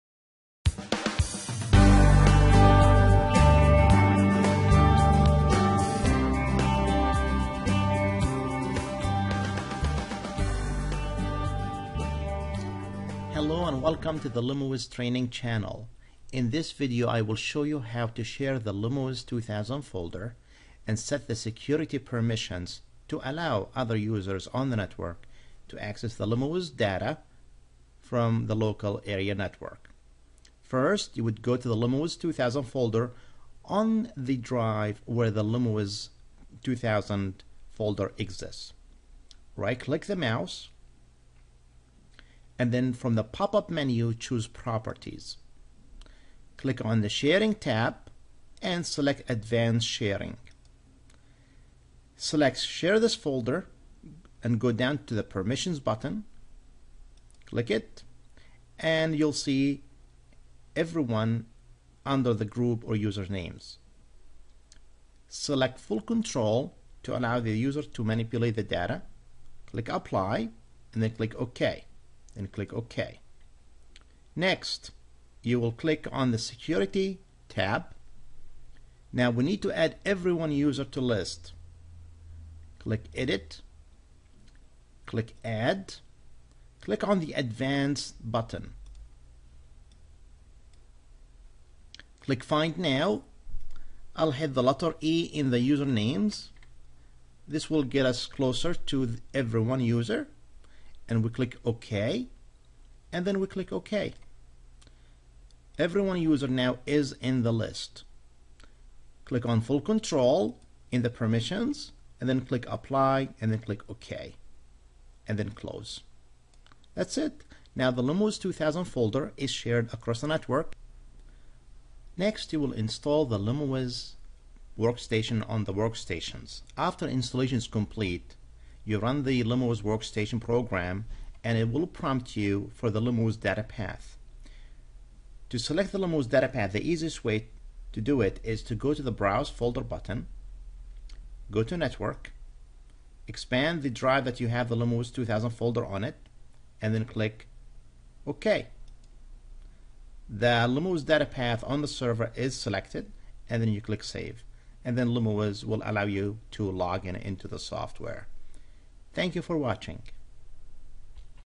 LimoWiz - Share LimoWiz Data Folder and Set Security Permissions
This video shows how to share the limowiz2000 folder on your local area network and configure the security permissions so that users can access the limowiz2000 folder.